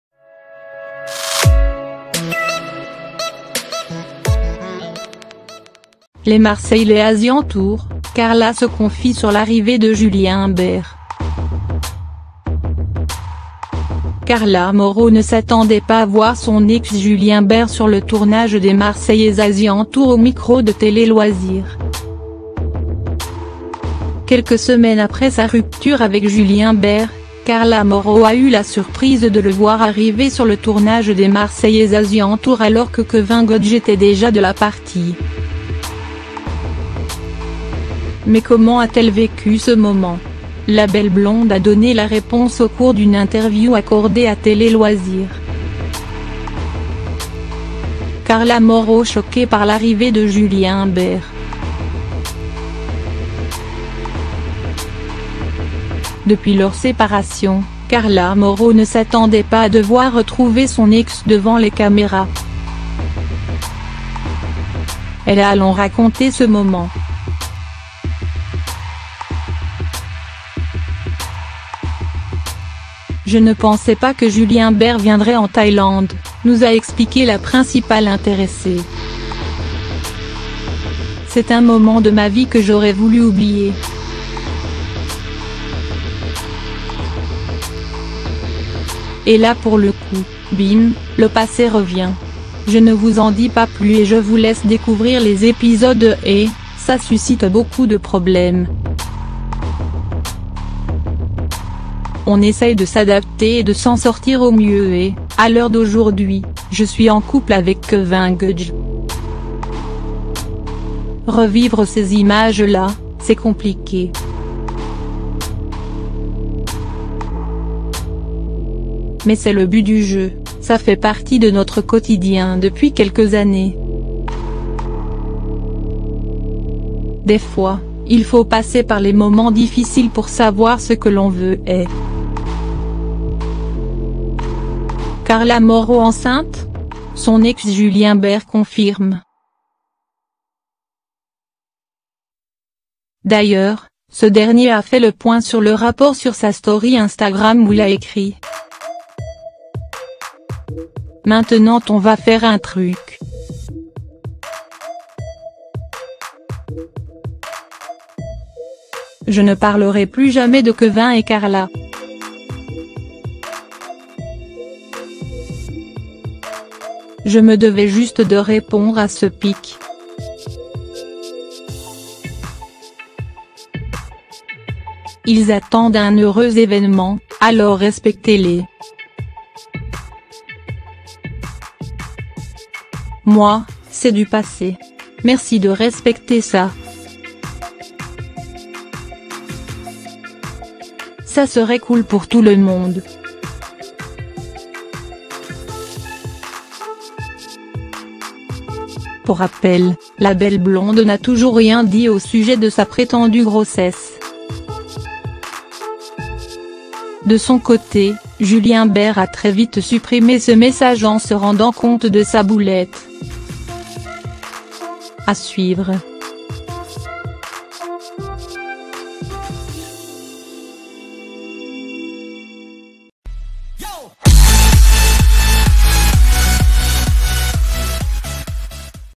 Les Marseilllais Asian Tour: Carla se confie sur l’arrivée de Julien Bert
Carla Moreau ne s'attendait pas à voir son ex Julien Bert sur le tournage des Marseillais Asian Tour au micro de Télé-Loisirs.